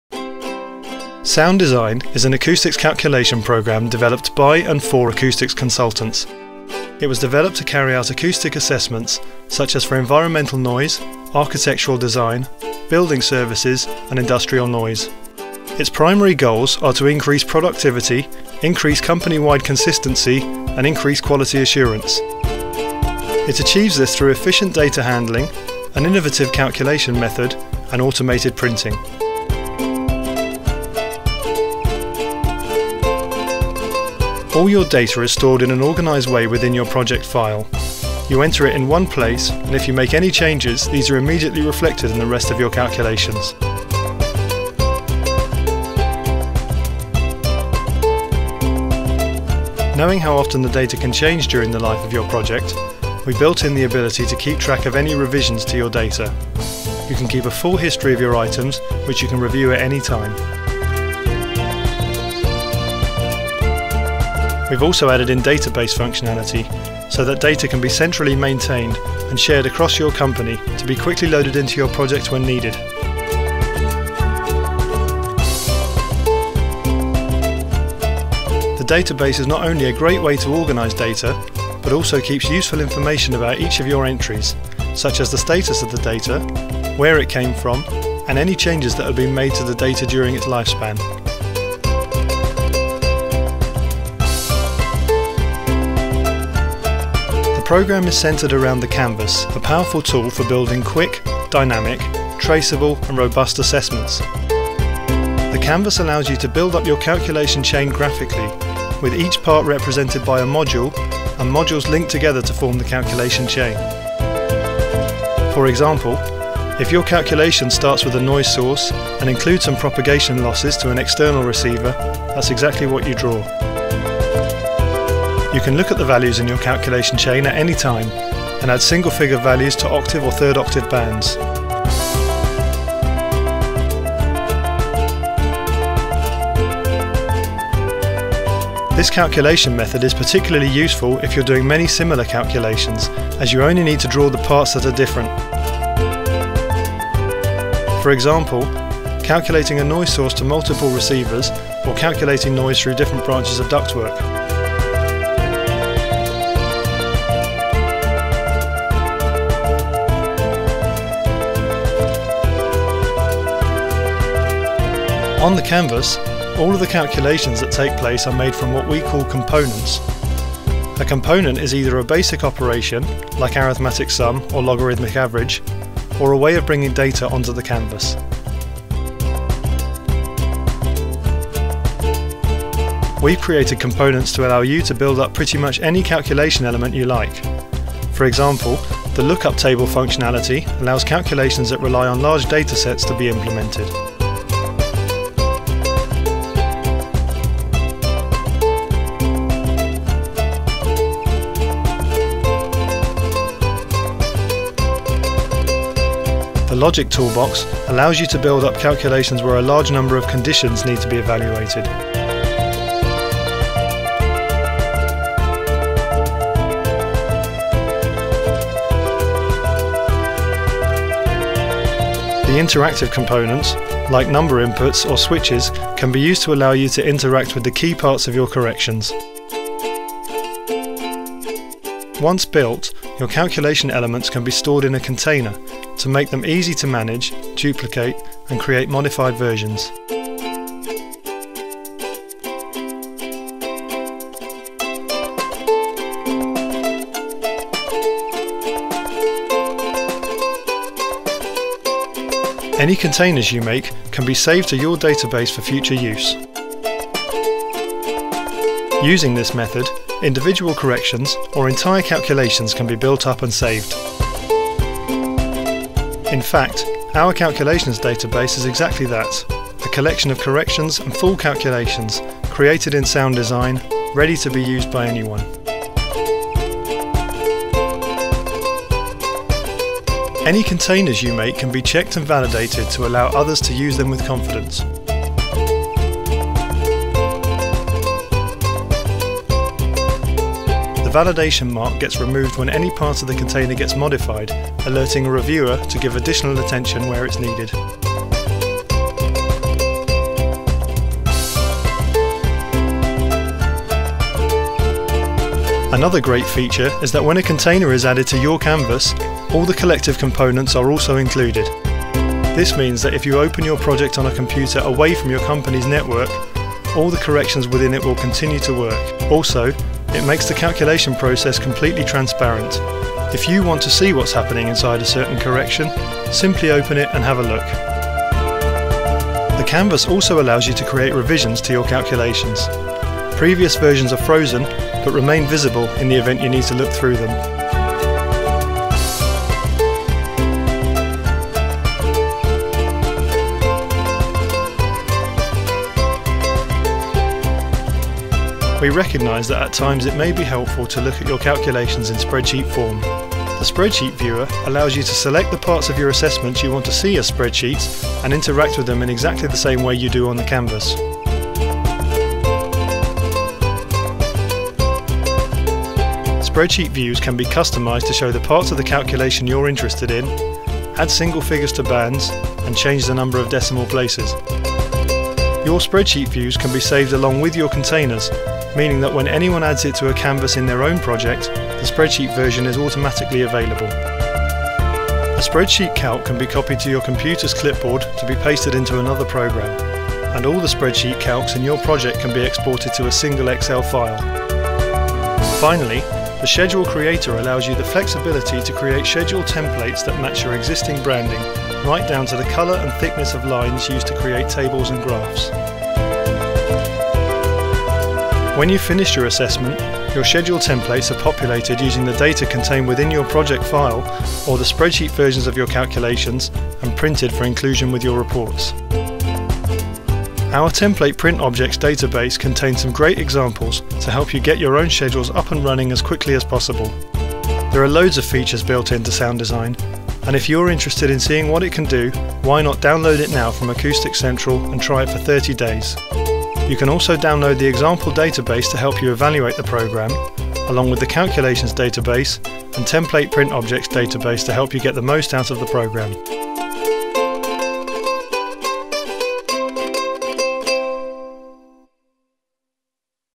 What is Sound Design
If you're an Acoustics Consultant, watch this video to find out what Sound Design is all about!